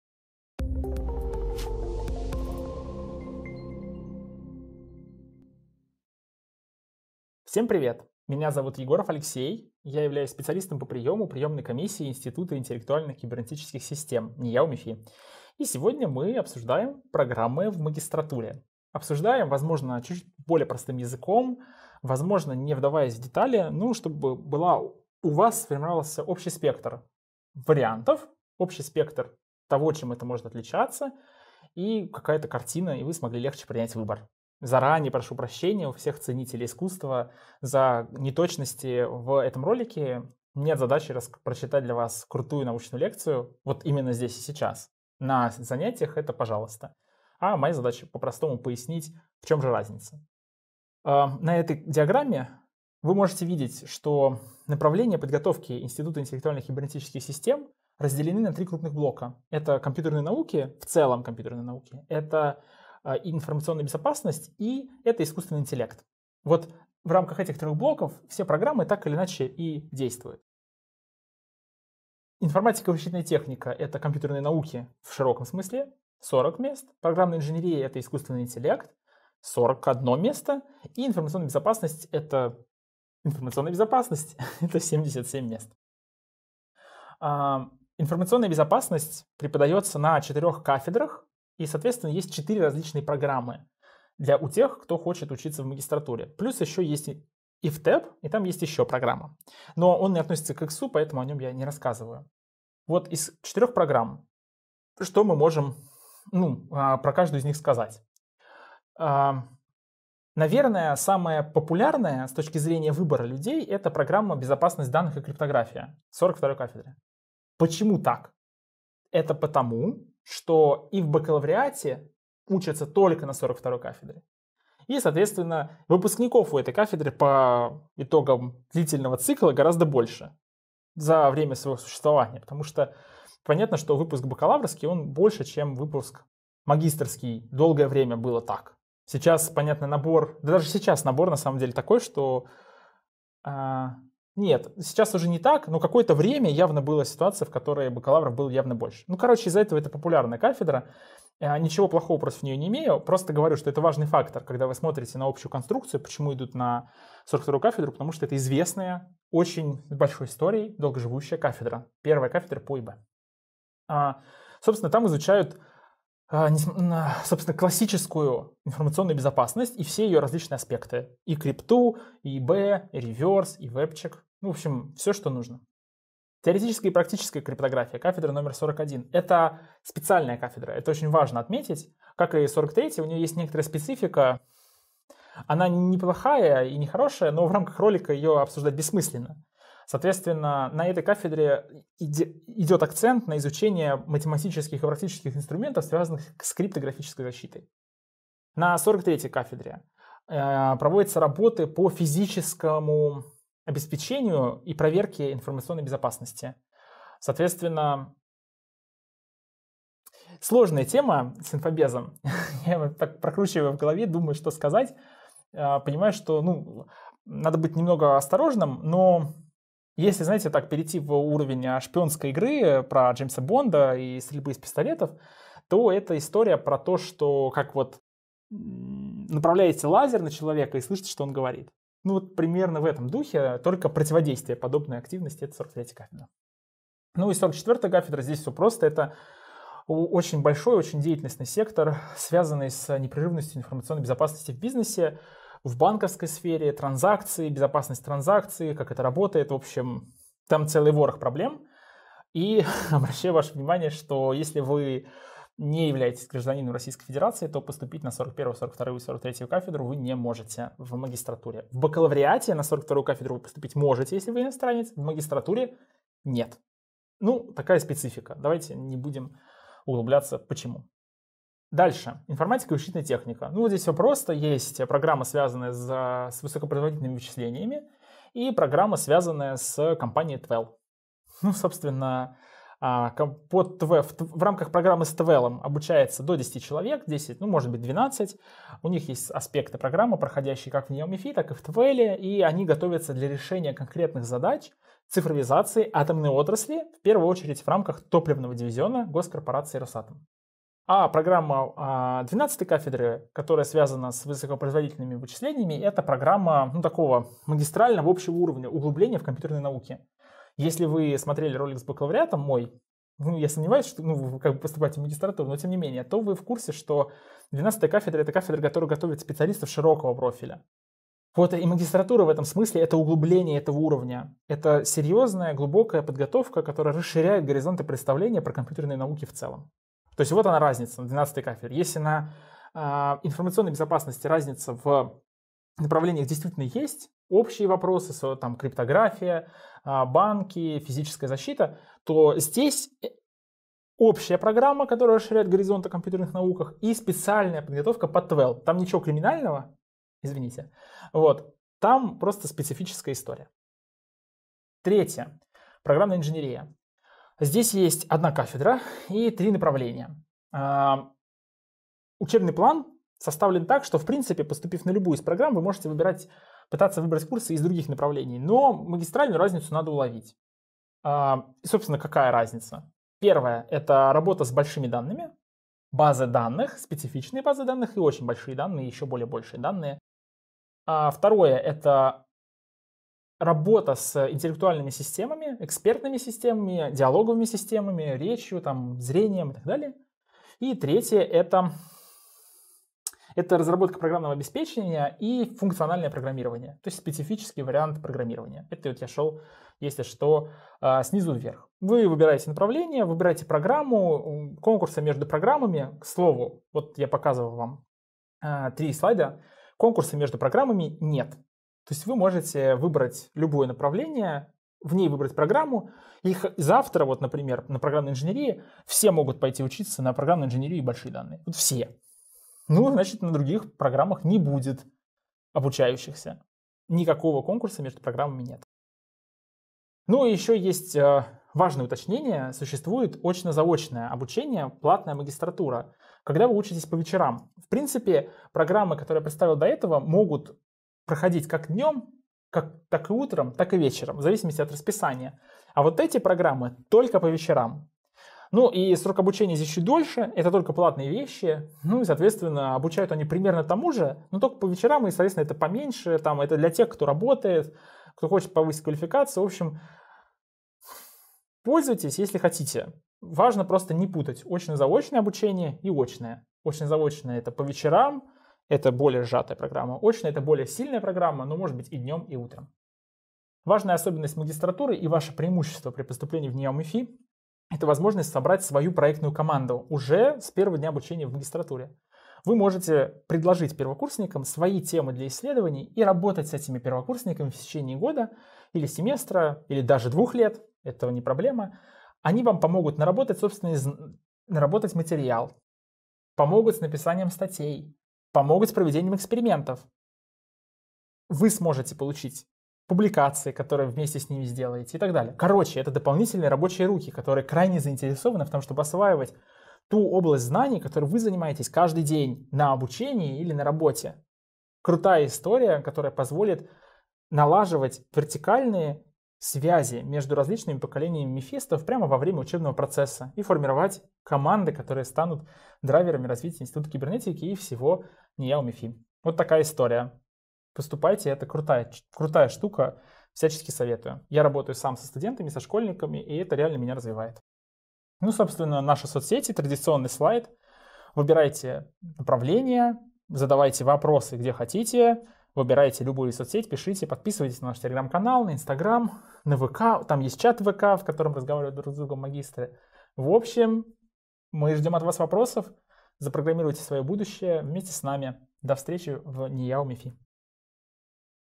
Поступай в магистратуру! / Образовательные программы / ИИКС
Поступай в магистратуру ИИКС!

Ведущий: Егоров Алексей

В видео разбираем различия между направлениями и реализуемыми на них программами. Рассказываем об уникальной возможности собрать собственную научную команду и получить бесценный опыт.

В видео:
00:00-00:55 Приветствие
00:56-01:10 Программы обучения
01:11-01:24 Распределение бюджетных мест
01:25-04:31 Информационная безопасность
04:32-05:53 Информатика и вычислительная техника
05:54-07:11 Программная инженерия
07:12-07:51 Очно-заочно обучение(платное)
07:52-09:22 Ведение проектной практики